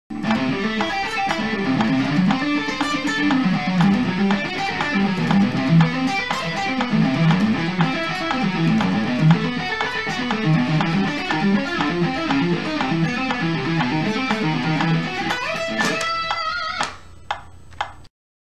Lick 45 "The Jazz Arpeggiator"
String skipping arpeggios in a ii-V-I progression.